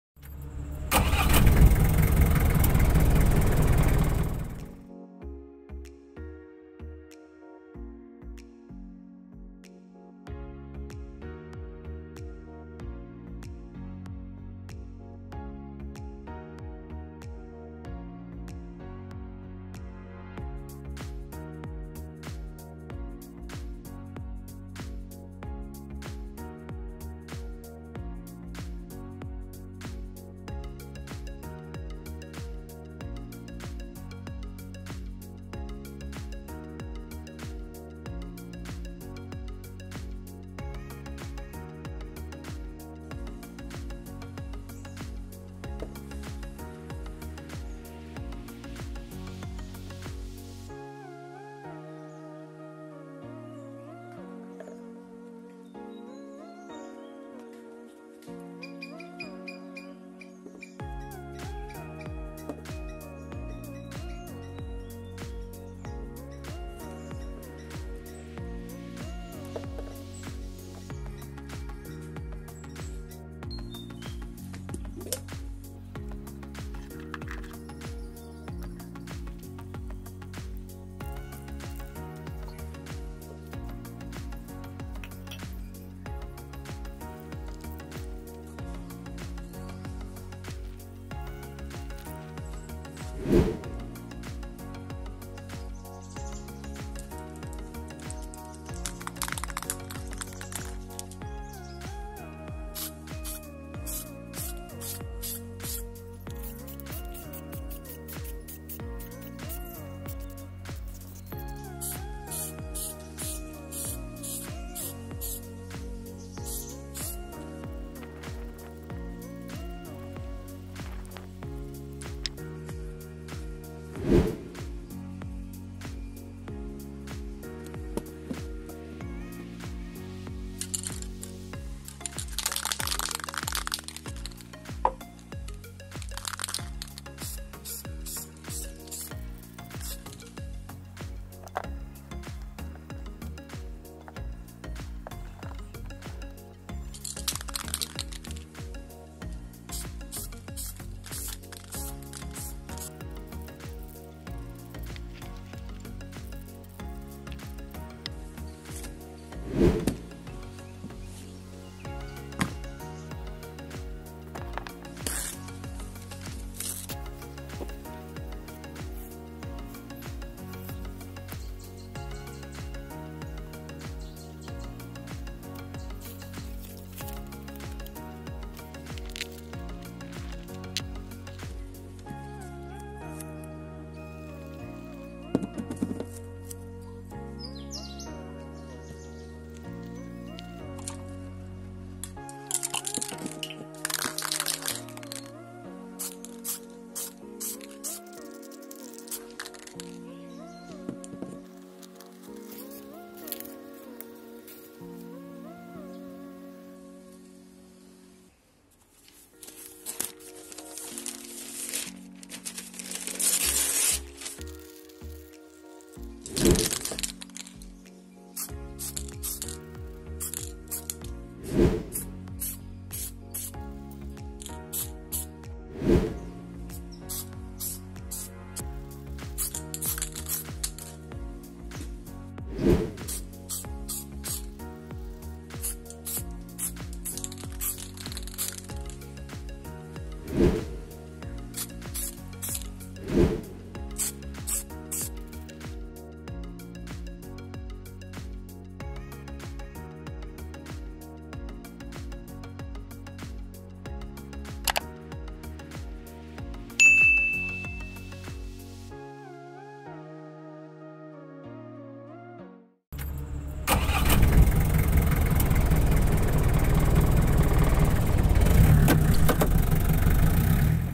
RC Landrover Defender | how to Paint | Paint Tricks | part 08 | dry tronic
🔴 Required Materials [ Ingredients ] - අවශ්‍ය ද්‍රව්‍ය [ අමුද්‍රව්‍ය ]

                   🔹 colour ( Spray bottle ) [ Black & Silver ]  - වර්ණ  ( ඉසින බෝතලයක් )  [ කළු සහ රිදී ]
                   🔹 Gray Normal Paint - අළු සාමාන්‍ය තීන්ත
                   🔹 News Paper - පත්තර කඩදාසි
                   🔹 Small Brush - කුඩා පින්සලක්
                   🔹 Gum Tape - ගම් ටේප් එකක්

⭕♻ DRY TRONIC | 💡📏📐
          The Tour is Filled with Dry Projects ...